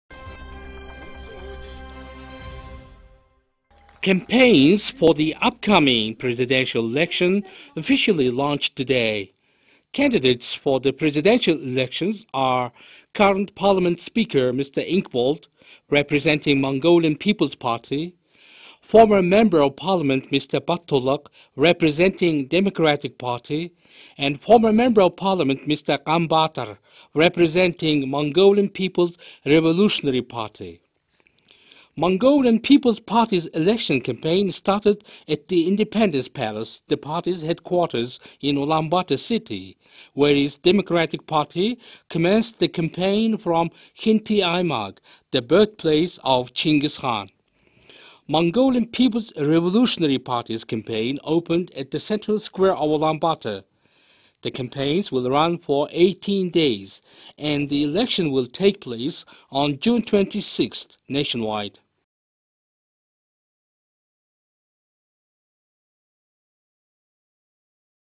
Election campaigns of presidential candidates launched
Campaigns for the upcoming Presidential election officially launched today. Candidates for the Presidential elections are current Parliament Speaker Mr M.Enkhbold, representing Mongolian People’s Party (MPP), former Member of Parliament Mr Kh.Battulga, representing Democratic Party (DP) and former Member of Parliament Mr S.Ganbaatar, representing Mongolian People’s Revolutionary Party (MPRP).
MPP’s election campaign started at the Independence Palace, the party’s headquarters in Ulaanbaatar city, whereas DP commenced the campaign from Khentii aimag, the birthplace of Chinggis Khaan. MPRP’s campaign opened at the central Square of Ulaanbaatar. The campaigns will run for 18 days and the election will take place on June 26 nationwide.